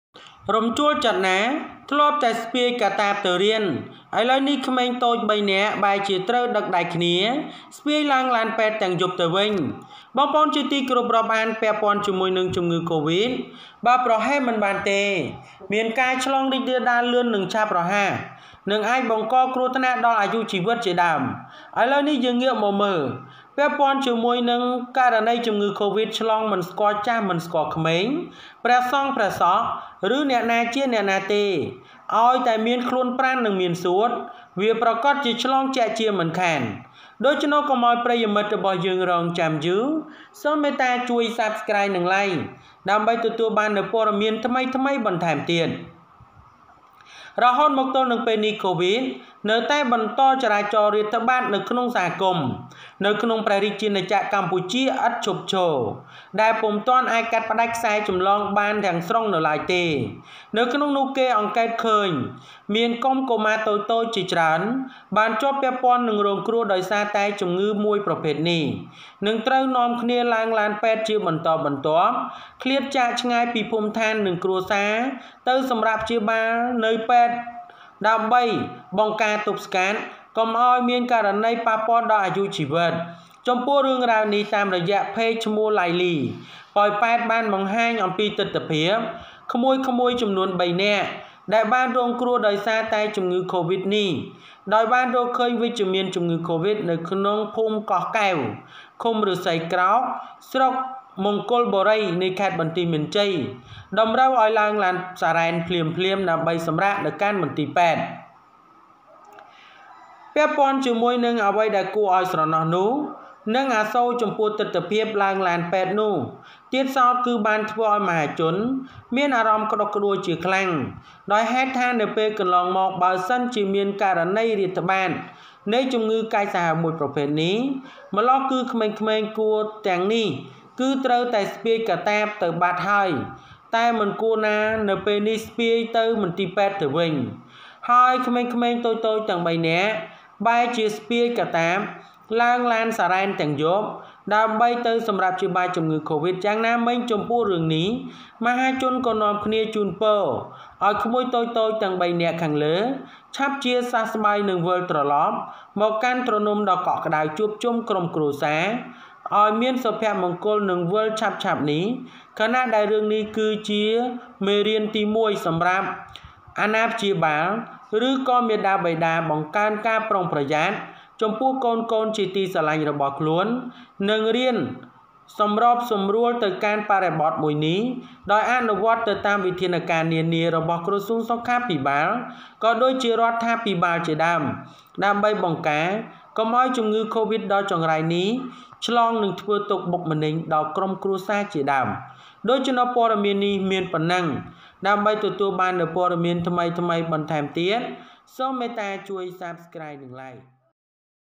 រំជួលចិត្តណាស់! ធ្លាប់តែស្ពាយកាតាបទៅរៀន ឥឡូវក្មេងតូច ៣នាក់បែរត្រូវដឹកដៃគ្នាស្ពាយឡើងឡាន
រំជួលចិត្តណាស់! ធ្លាប់តែស្ពាយកាតាបទៅរៀន ឥឡូវក្មេងតូច ៣នាក់បែរត្រូវដឹកដៃគ្នាស្ពាយឡើងឡាន Very emotional!  Used to carry backpacks to school, now 3 small children have to carry each other in the car.